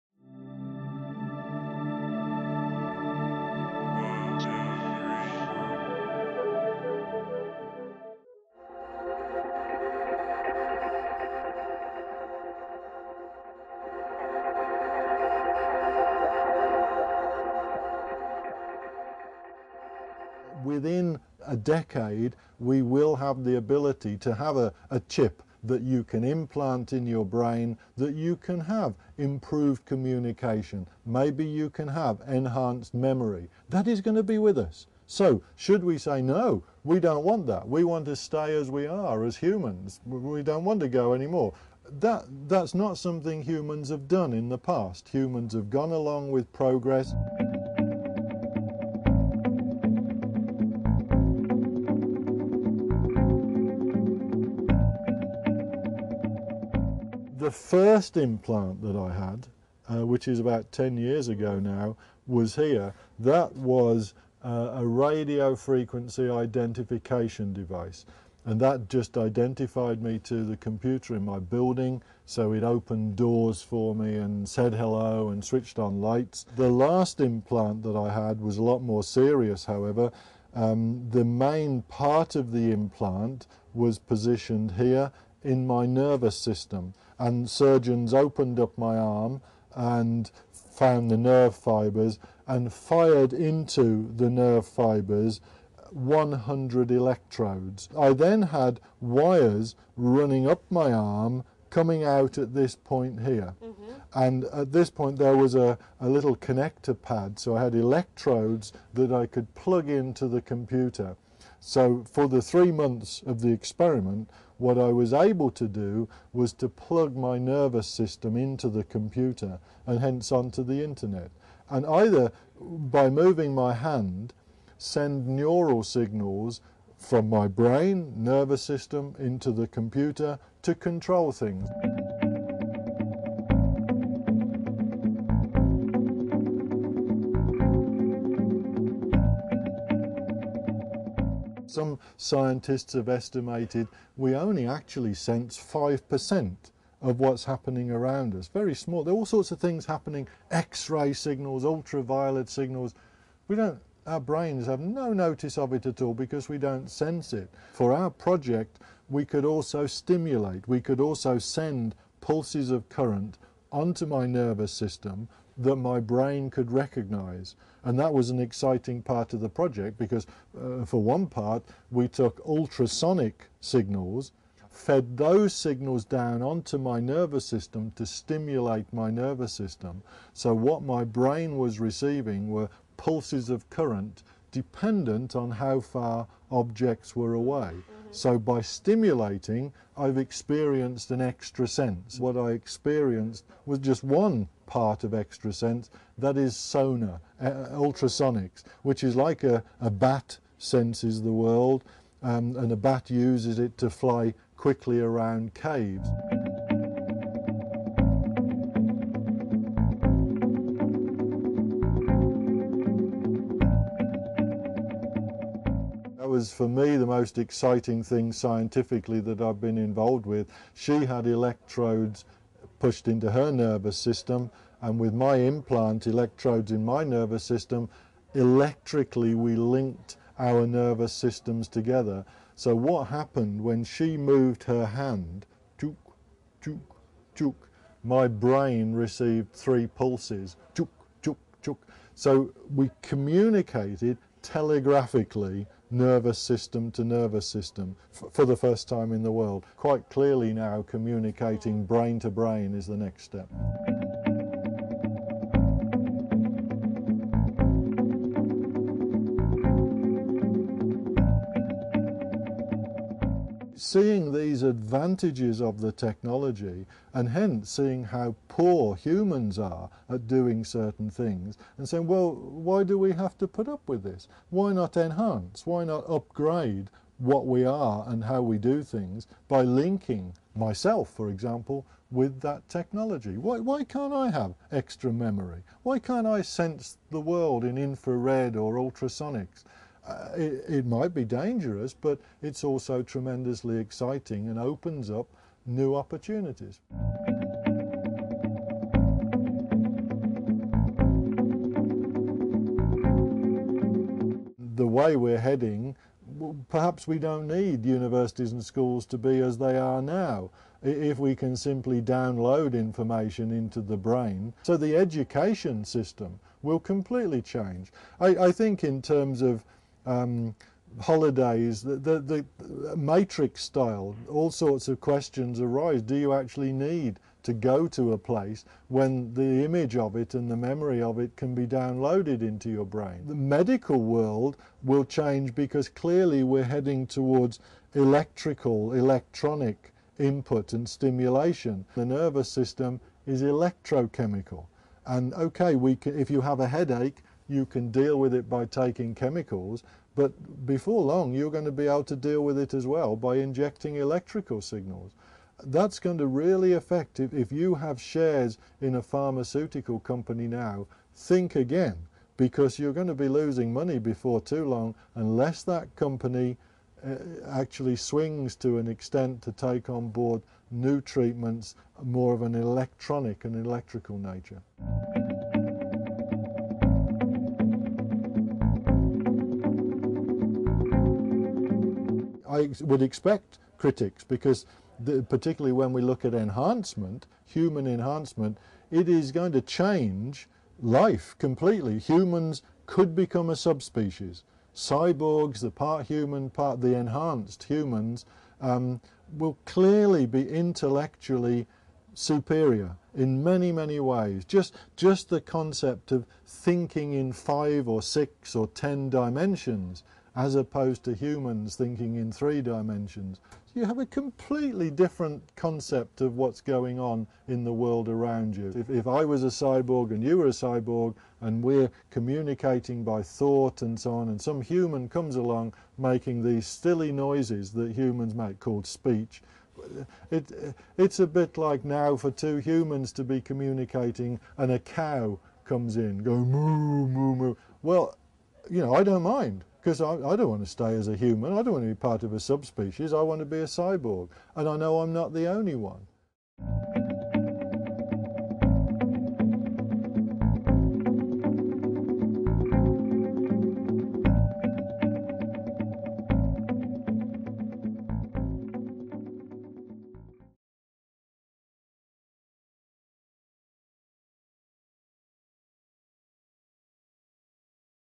CYBORG LIFE - Talking to Kevin Warwick
Will the human race be replaced by cyborgs anytime soon?
In 1998, when Kevin Warwick, Professor of Cybernetics at the University of Reading, implanted a silicon chip transponder into his left arm and connected it to his nervous system, he became the world's first cyborg: a man-machine hybrid. In this video interview we talk about brain-to-brain telepathic communication, the therapeutic benefits of his experiments and why he won't be the only cyborg on this planet in the future.